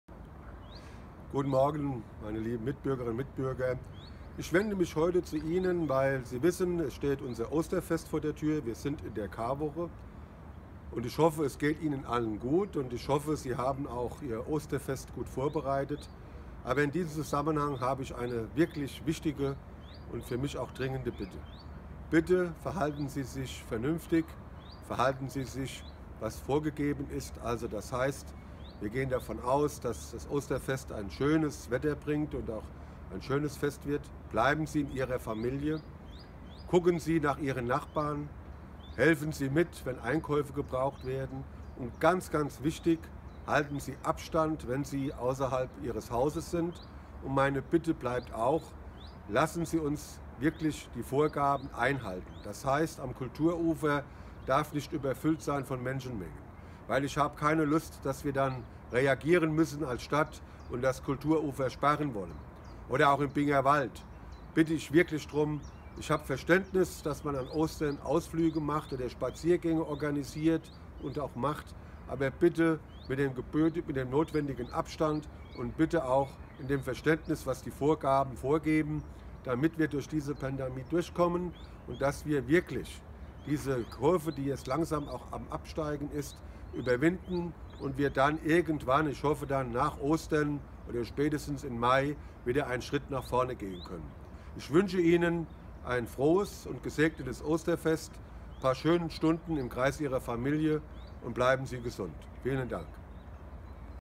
OB Feser – Ostergrüße und Bitte um Einhaltung der Vorsichtsmaßnahmen
Der Binger Oberbürgermeister Thomas Feser wünscht allen Bürgerinnen und Bürgern ein frohes Osterfest. Darüber hinaus ruft das Stadtoberhaupt aber auch zur Besonnenheit und zum Einhalten der aktuellen Maßnahmen im Zusammenhang mit der Corona-Pandemie auf.